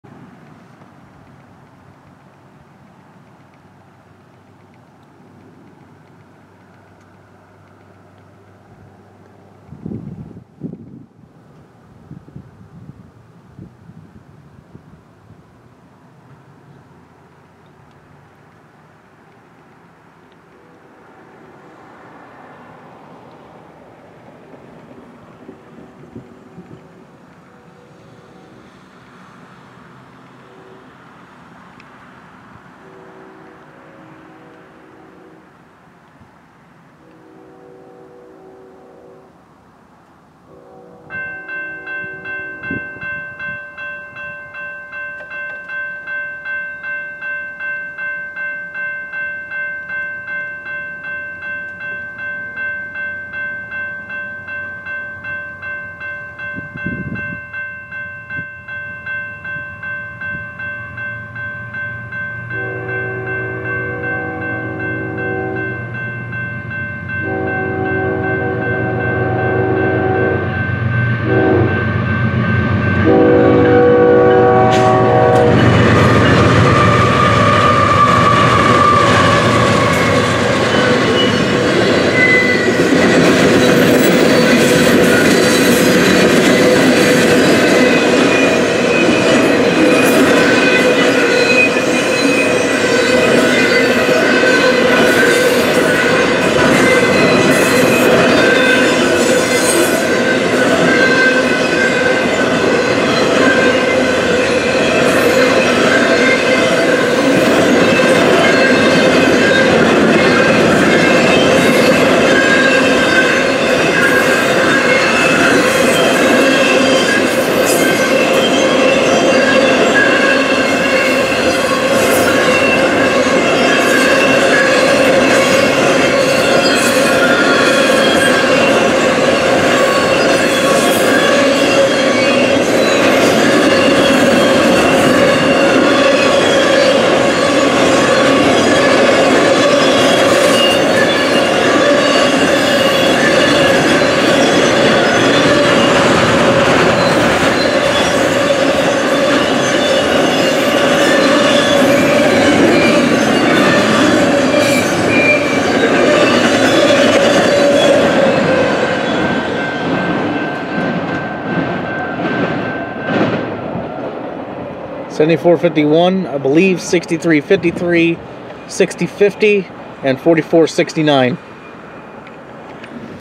Weak K5HL on BNSF 7451 East Coal City, IL 12/3/18
EB Hotshot, 5th Ave., Coal City, IL, BNSF Chillicothe Sub
BNSF 7451 = ES44DC - H3 - Weak Raised Letter K5HL
BNSF 6353? = ES44AC - H3
BNSF 6050 = ES44AC - H3